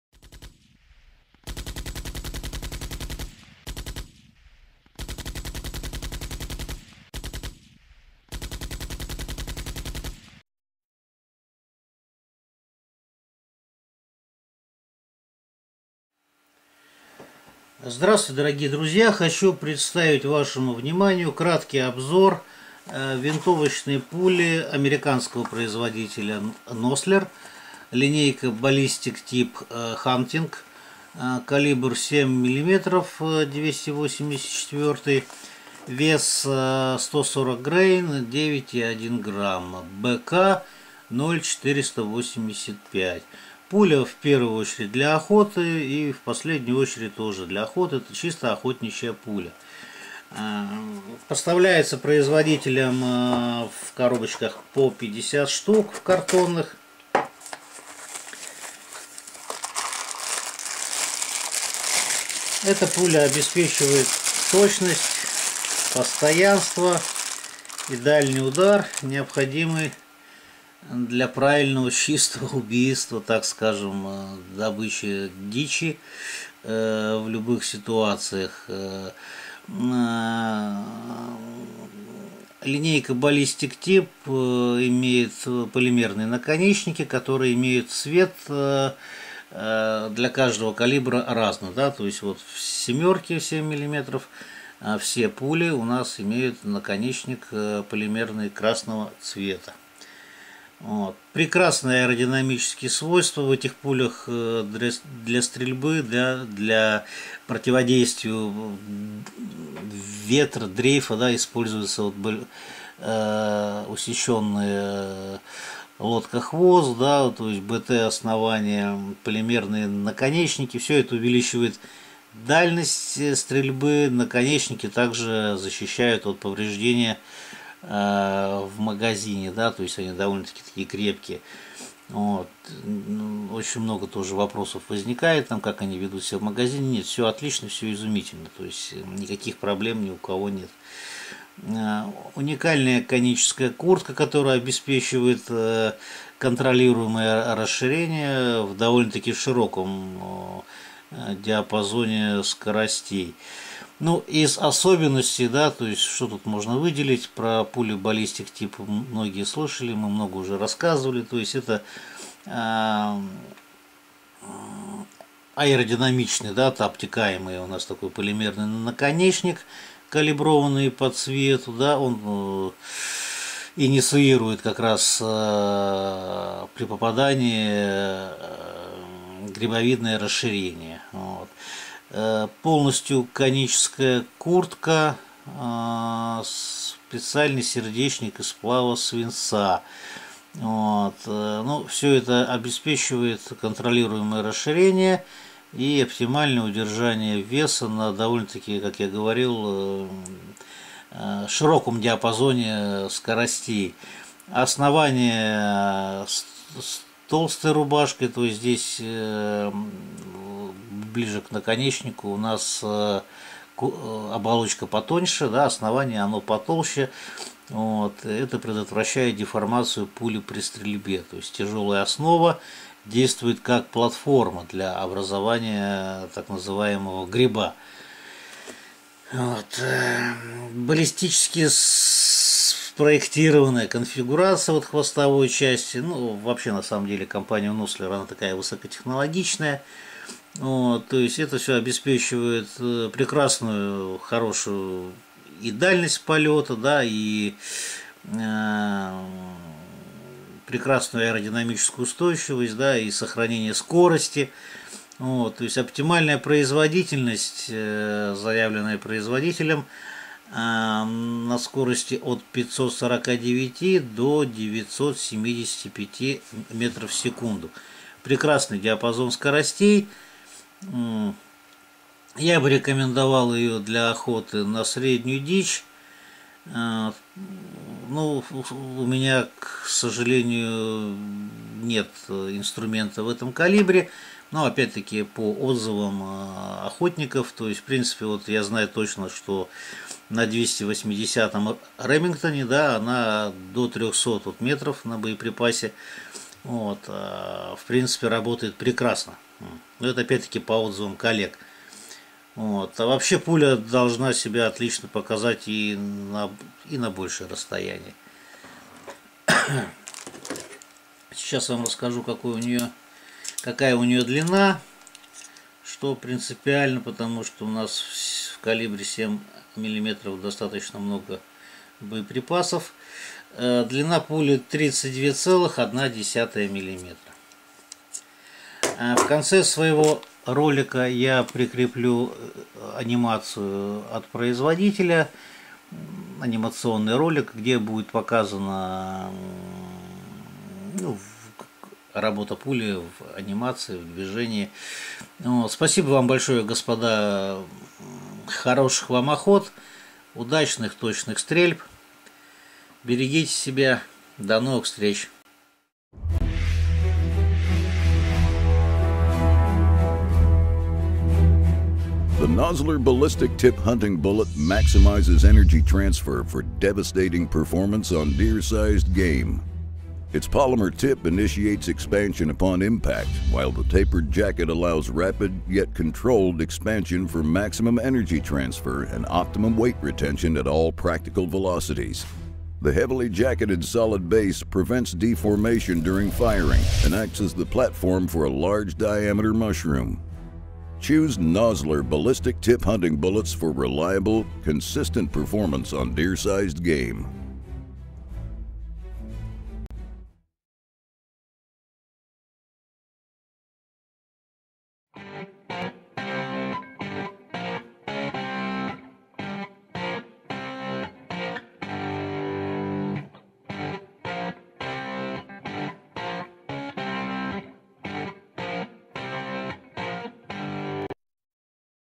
Nosler Ballistic Tip 7мм/.284 140 gr/9,1 грамм, BC-0,485.
Обзор пули для охоты Nosler Ballistic Tip  7мм/.284 140 gr/9,1 грамм, BC-0,485.
Описание, внешний вид, характеристики
ТОВАРЫ С ALIEXPRESS, КОТОРЫЕ МОГУТ ПРИГОДИТСЯ. ТОВАР И ПРОДАВЕЦ ПРОВЕРЕНЫ ЛИЧНО.